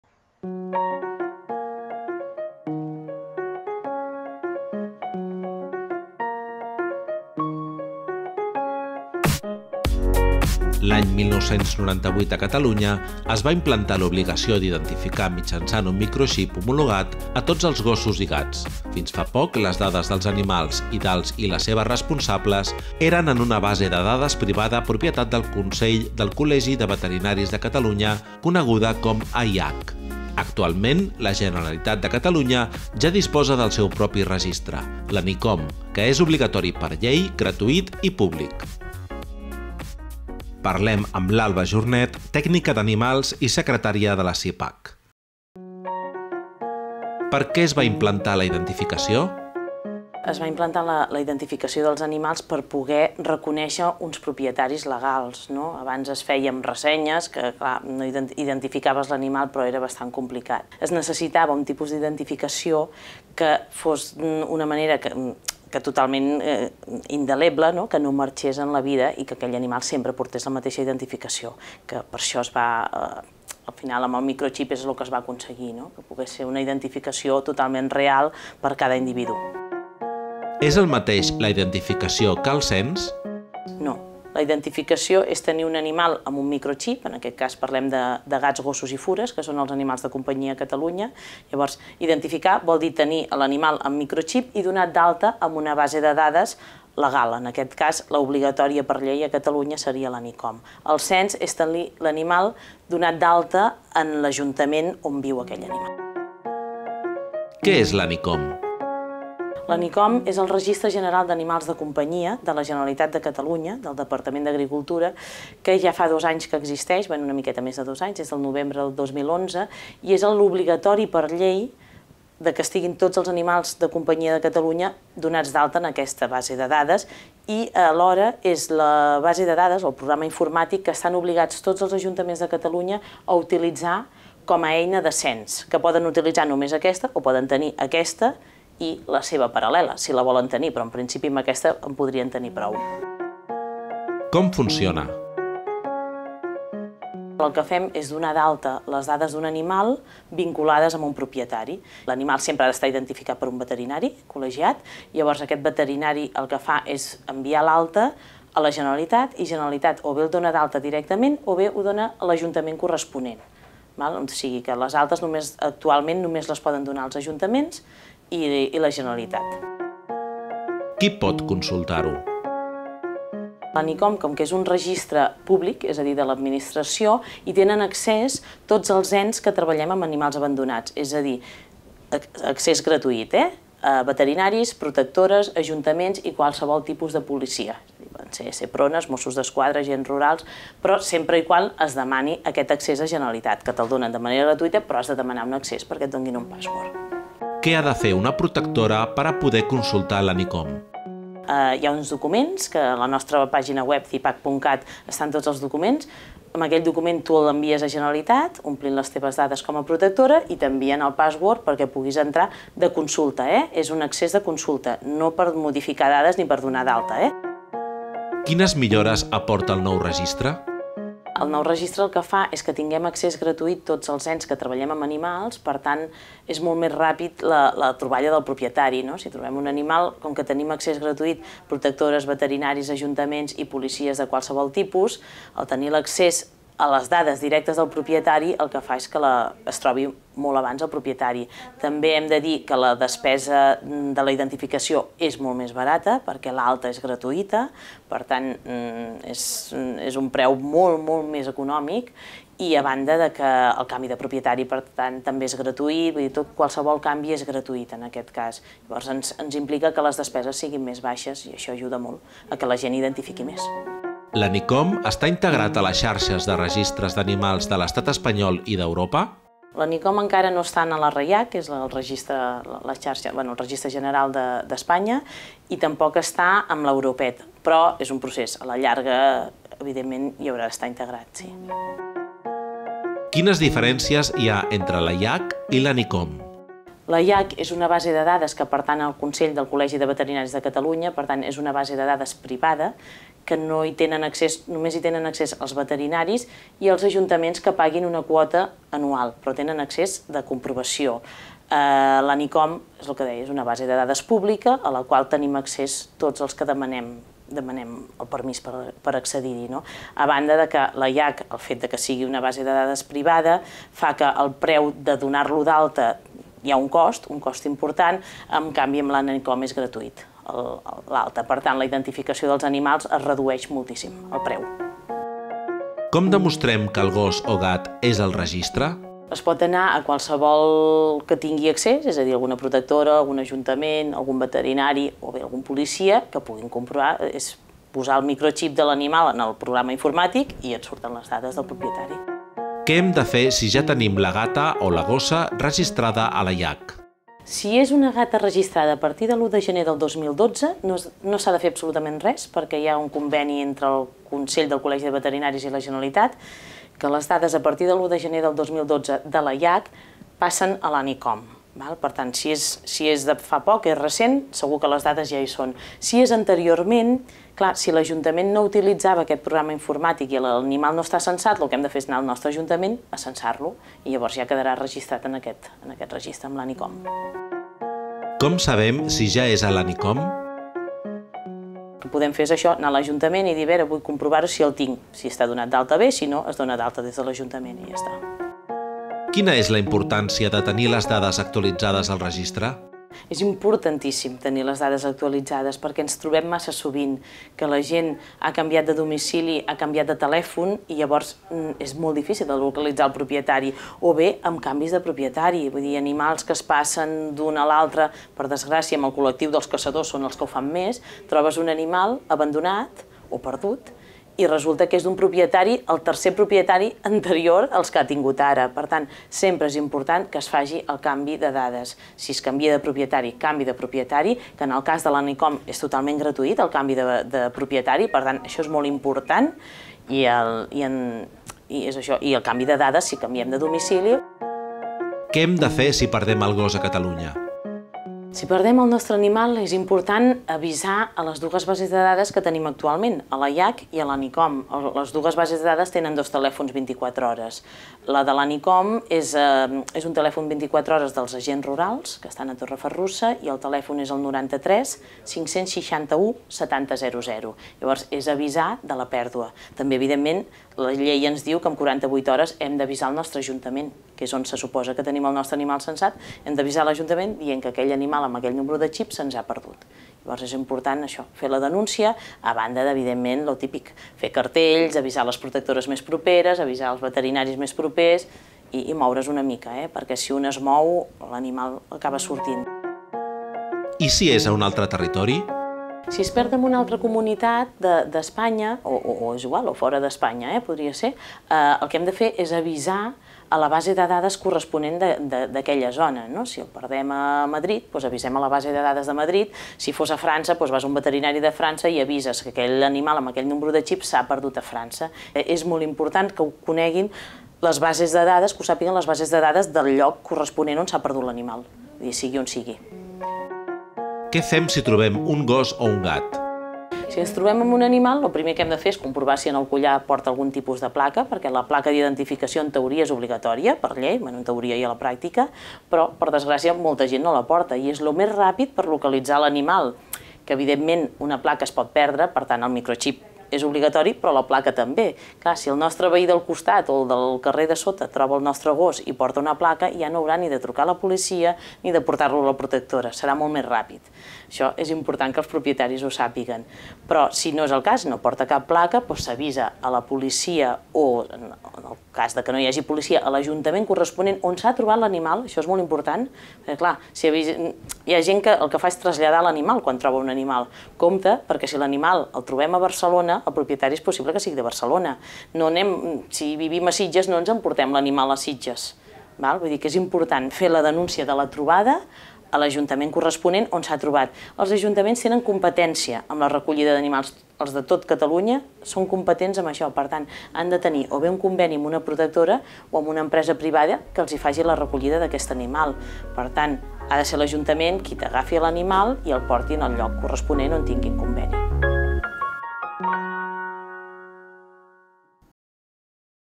[cat] L'ANICOM, el registre públic d'animals a Catalunya
Mira tots els nostres programes a TVAnimalista.com

L’any 1998 a Catalunya es va implantar l’obligació d’identificar mitjançant un microxip homologat a tots els gossos i gats. Fins fa poc les dades dels animals i dels i les seves responsables eren en una base de dades privada propietat del Consell del Col·legi de Veterinaris de Catalunya coneguda con AIAC. Actualment la Generalitat de Catalunya ja disposa del seu propi registre, l’ANICOM, que és obligatori per llei, gratuït i públic.